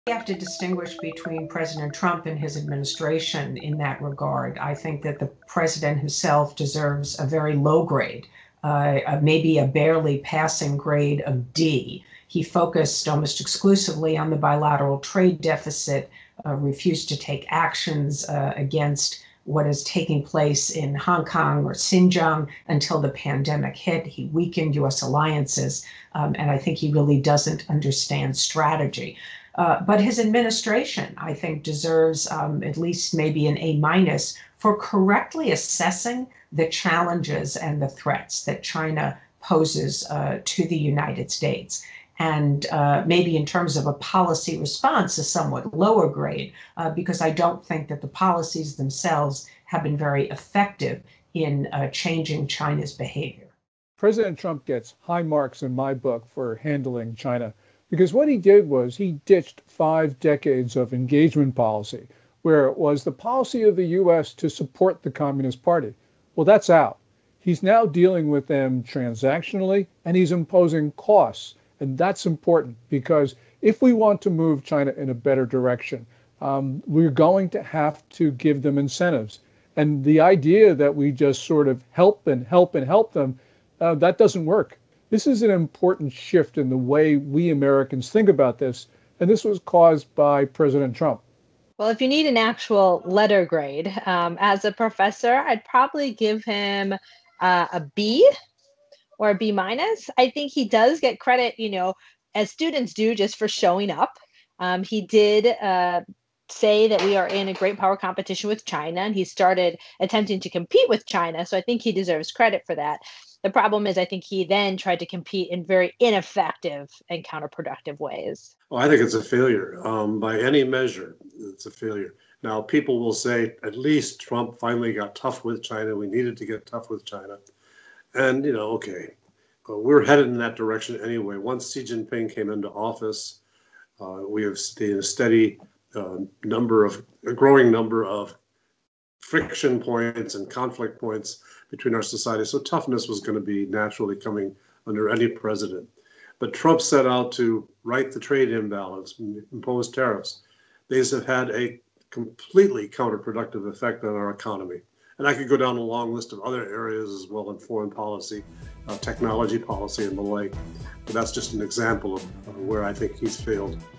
【特朗普对华政策成绩单 美国学者打几分？】1/18
（葛来仪）：我们必须把特朗普总统本人和特朗普政府区分开来。我会给总统本人一个很低的分数，也许是勉强及格的D。 他几乎只关注双边贸易赤字，而拒绝对香港或新疆发生的事情采取行动，直到新冠疫情发生。他削弱了美国的同盟关系，我认为他真的不懂策略。但至于特朗普行政当局，我认为至少也许应得A-，他们正确评估了中国对美国的挑战和威胁，也许在政策应对方面分数要低一点，因为我不认为（特朗普政府的）政策本身对改变中国行为非常有效。

（章家敦）：在处理中国问题方面，我给特朗普总统打很高的分数，因为他抛弃了过去五十年来的对华接触政策，那是美国支持中国共产党的政策。那样的政策已经不复存在了。他现在是以交易的方式和他们打交道，而且他增加了（打交道的）成本。这一点很重要，因为如果我们想推动中国向更好方向发展，我们就必须给他们向那个方向发展的诱因。如果我们只是一味帮助、帮助、再帮助他们，是行不通的。那听起来好像会有用，但并不是这样的。所以，这是我们美国人思考这个问题的一个重要转变，而这要归功于特朗普总统。

（梅惠琳）：如果真要打分数，作为教授，我可能会给他一个B或B-。我认为他确实有功劳。就像学生一样，你出勤就会得分。他确实说过，我们正在与中国进行大国竞争，他也开始尝试与中国竞争。我认为在这点上他是值得肯定的。我认为问题是他后来试图以非常无效的、适得其反的方式进行竞争。

（包道格）：我认为无论用什么方法评估都是失败的。 人们会说，至少特朗普终于对中国强硬起来了。 我们需要对中国采取强硬态度。 你知道，我们本来就朝着这个方向前进。习近平上任后，我们两个社会之间的摩擦点和冲突点越来越多，因此不管是哪位总统执政，都自然会对中国强硬起来。特朗普上任后着手纠正贸易不平衡问题，开始征收关税。 这些都对我们经济产生完全适得其反的作用。
相关视频
时事大家谈: 特朗普四年对华政策 重量级美国学者如何评分？

**通过Google Podcasts收听美国之音**

**收听收看美国之音中文电视、广播节目**